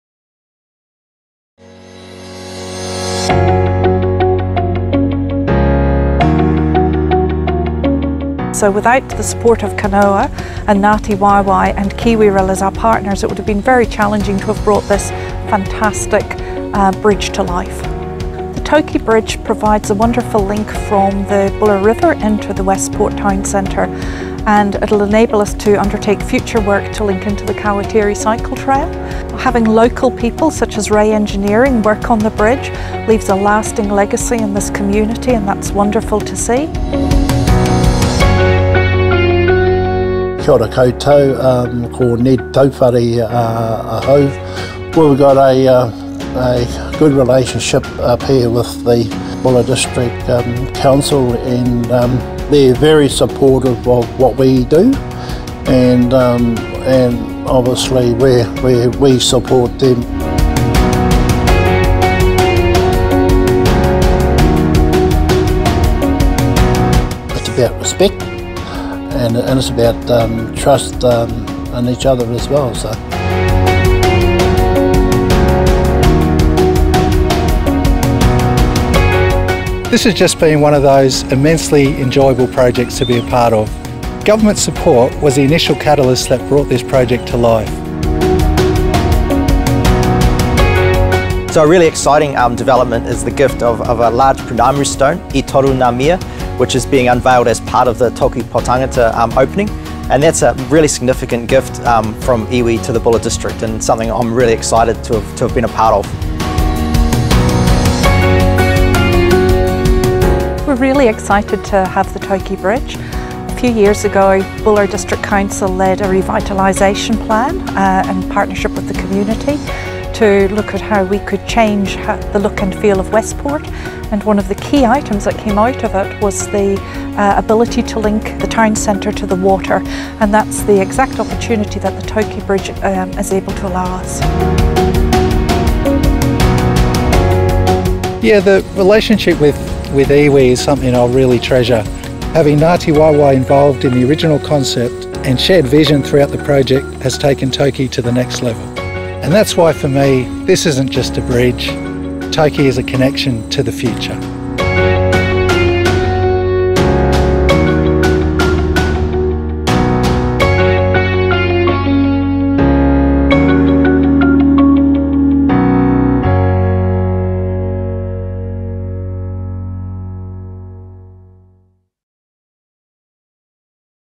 The Toki Poutangata bridge comes to life
Watch the making of the Toki Poutangata bridge and listen to the incredible story of strong partnerships, local craftsmanship, and a gifted treasure that is behind this ambitious project.

Since 2020 craftsmen, designers, engineers, contractors, and Council staff worked on the Toki Poutangata bridge.

The $2M project, funded mainly from the Provincial Growth Fund, required a diverse team to turn it into reality. Championing innovation in design, incorporate new technologies, finding unique engineering solutions, and utilising the skill of local contractors, Toki came to life.

Credit Nomad Audio & Video & also Auckland Libraries Heritage Collections.